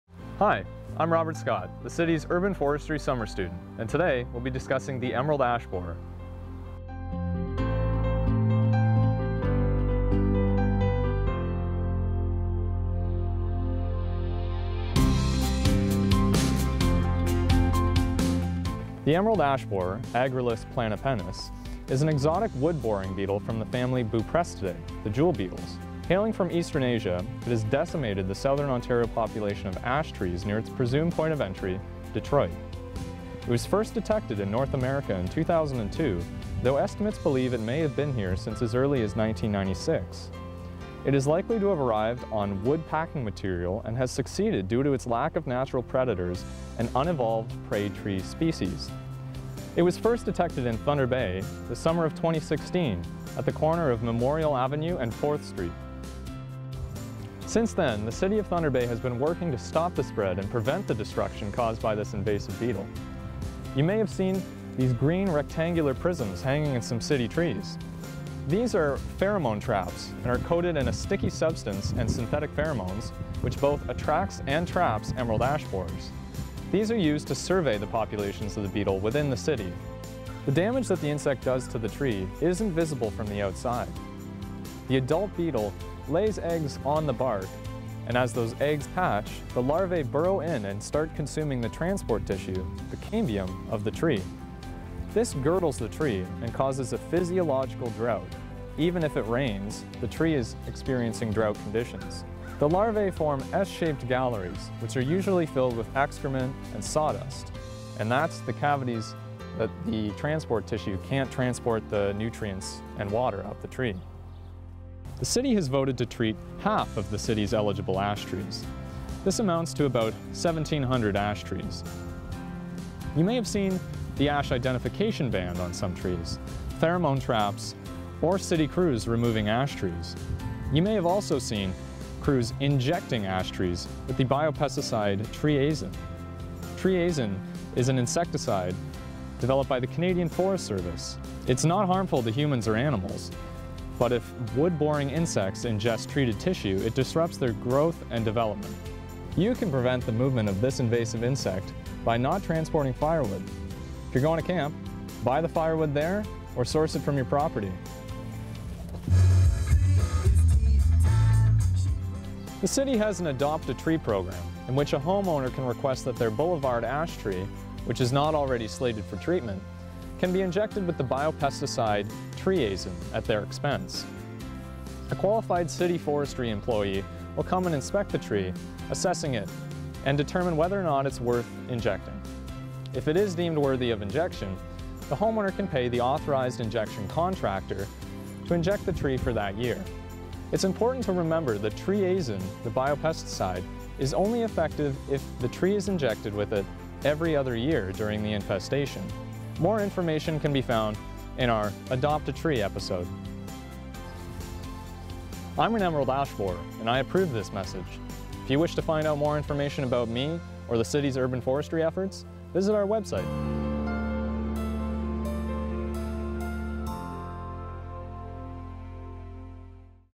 Urban Forestry | Episode 2 - Emerald Ash Borer
Taking a look into the world of Trees. Robert Scott, City of Thunder Bay Urban Forestry Summer Student, talks about how the city is dealing with Emerald Ash Borer.